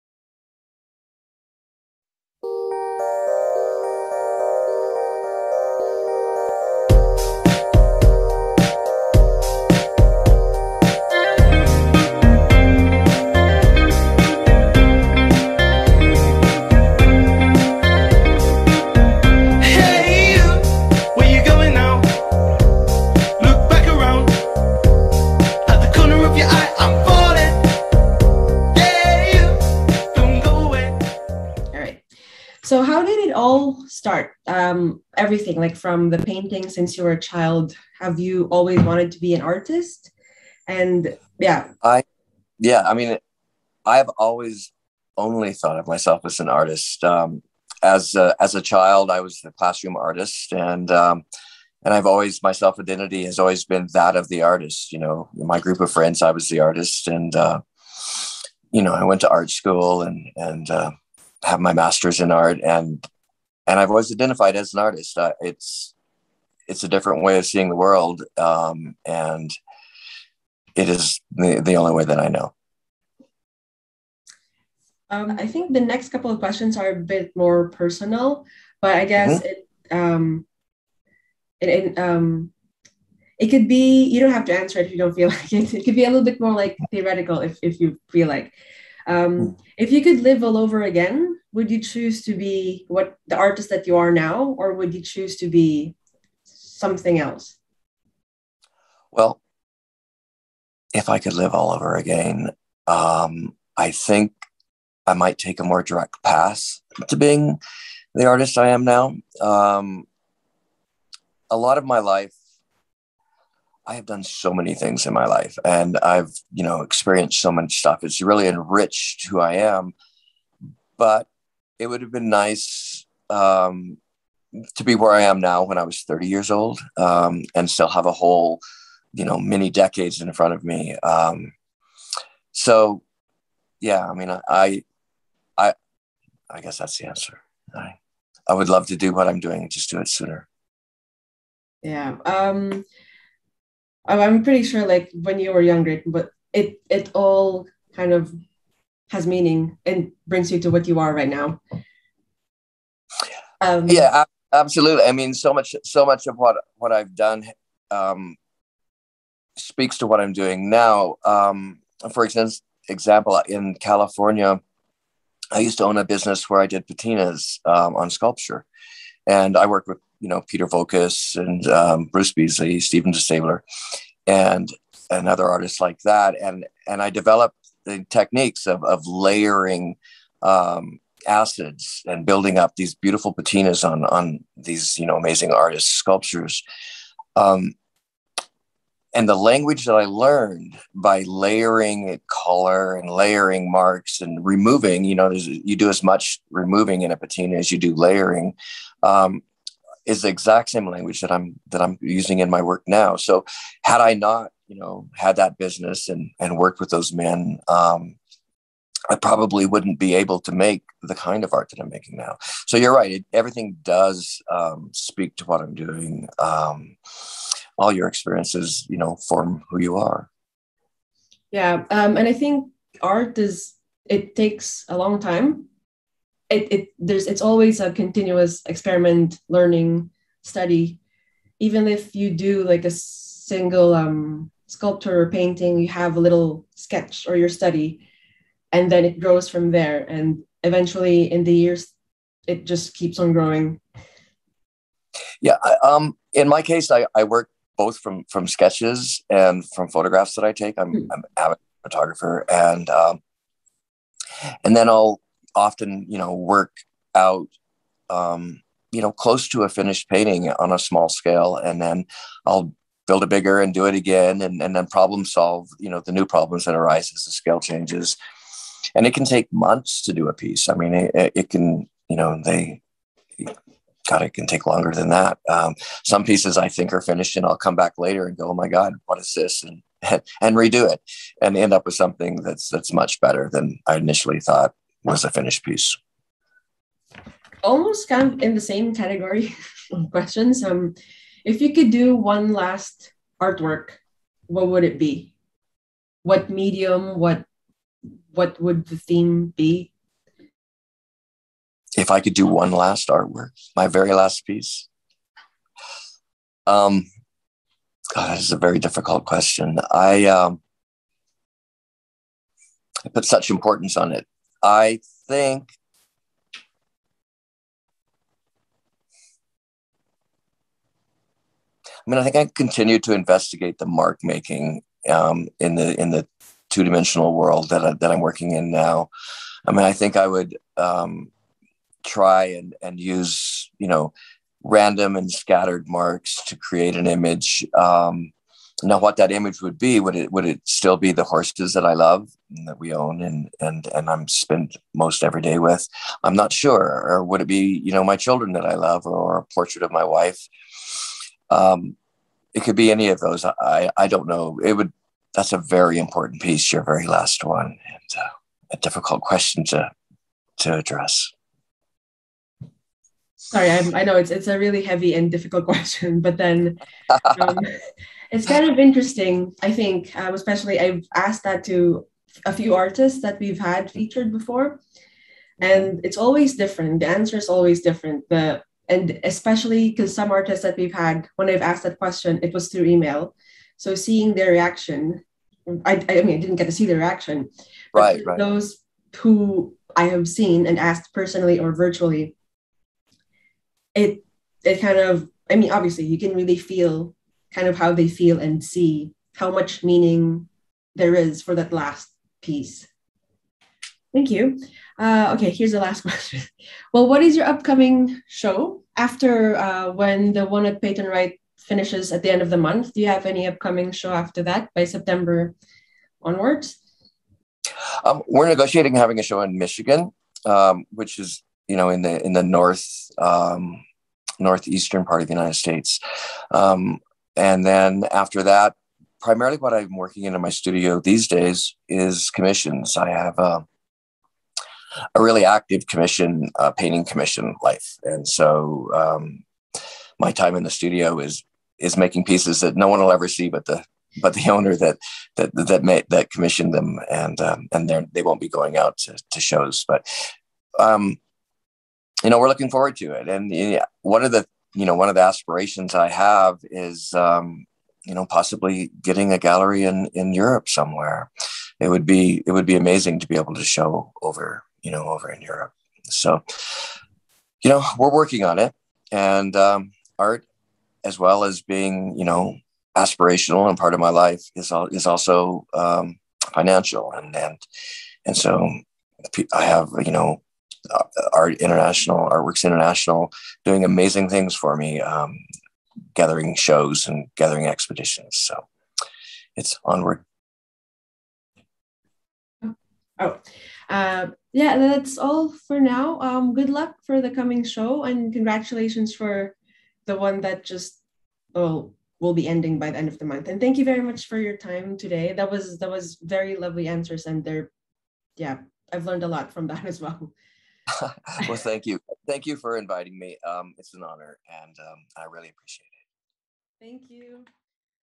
Lex Lucius of Artwork International
Printmaker, Painter, Sculptor

Bachelor of Fine Art, CCA, 1985 | Master of Arts, University of New Mexico, 1989

“I live in the Roaring Fork valley just north of Aspen Colorado, tucked into the Rocky Mountains. My life is full of family, painting and horses and my clothes smell of the stable, and on far too many days my boots of the pasture. Less than five minutes from my painting studio is the stable where my wife Aimée keeps her jumping horses and my daughter her pony. When I drive over watch them ride, which I do several times a week I pass by a field of polo ponies. It’s these ponies that have become my favourites to paint because I love their small muscled bodies and I see such strength and determination in their movements. At the stable our warmbloods are huge muscled yet incredibly calm animals, even in my paintings they have a sureness of movement and a stillness that speaks of this confidence.

I try to invoke the feelings I get from these animals but just as importantly I also try to bring the stories and dreams we all carry within us when we think of horses and what horses mean to us all. I am focusing on art I want to see, art that makes me feel. It is my hope that these paintings bring out feelings of comfort and connection in the viewers also.”

-Lex Lucius Artist Statement

(c) Images and information courtesy of the artist and Artwork International